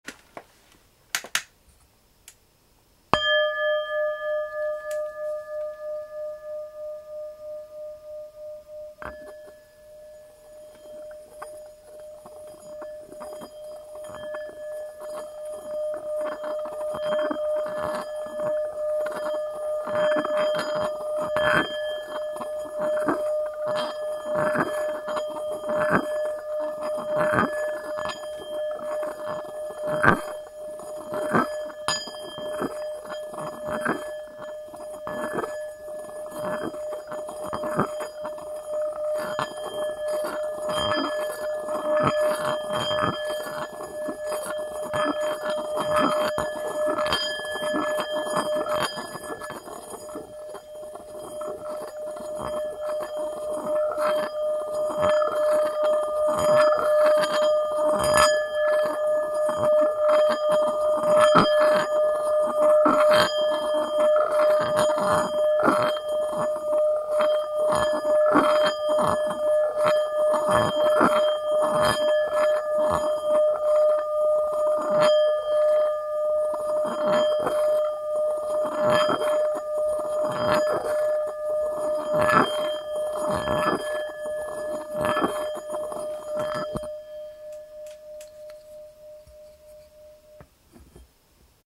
Tibetan Singing Bowl
The mallet causes vibration from dinging, and circling the bowl creates a consistency in the vibration, permitting the bowl to "sing." These are called standing bells as the initial purpose was precisely that, and the singing pattern is intended to induce a calm state suitable for meditation. With proper practice I should be able to eliminate the grinding noise of the mallet, by keeping consistent and pressing more firmly upon the bowl's surface.